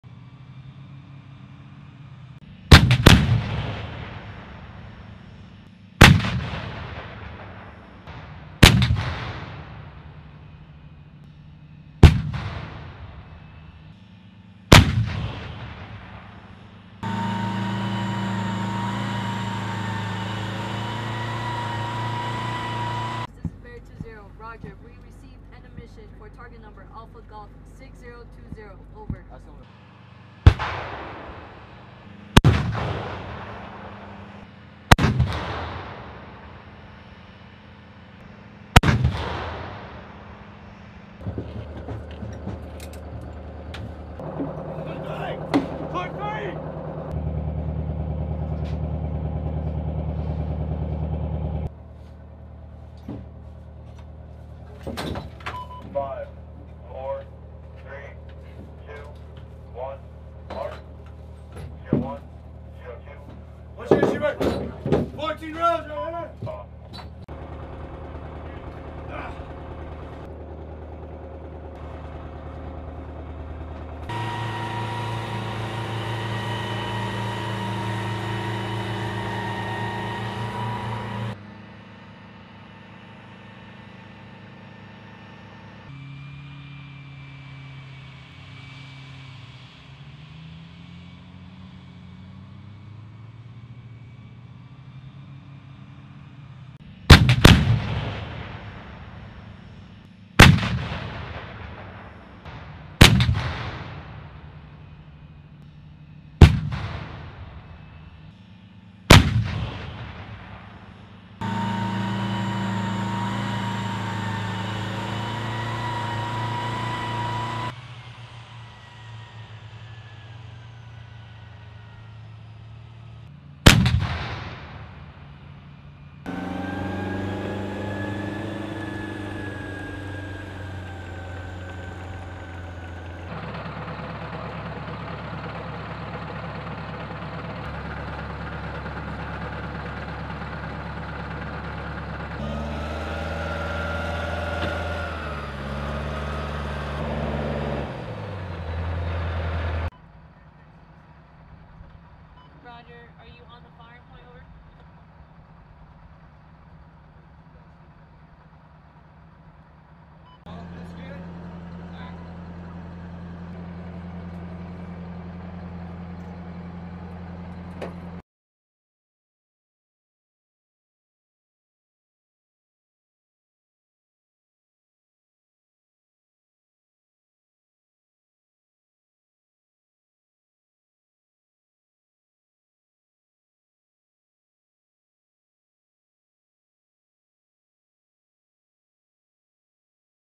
Bravo "Bear" Battery, 4-27 FA Commence Artillery Table XV
Full Force Artillery Excellence: Bravo "Bear" Battery in Action at Forward Operating Station Torun, Poland - Artillery Table XV
Bravo "Bear" Battery, 4th Battalion, 27th Field Artillery Regiment, 2nd Armored Brigade Combat Team, 1st Armored Division, commenced battery qualification, Artillery Table XV at Forward Operating Station Torun, Poland February 13-16, 2024. During Artillery Table XV, Soldiers conduct tactical movements from several firing points and process multiple distinct fire missions.

TORUN, POLAND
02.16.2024

It is battery level training exercise that integrates fires into a tactical training scenario evaluated by senior FIRES leadership and master gunners to qualify the battery and ensure they can provide accurate and timely fires in support of the brigade. (Video by U.S. Army Spc. Trevares Johnson)

Film Credits: Video by Spc. Trevares Johnson
2nd Armored Brigade Combat Team, 1st Armored Division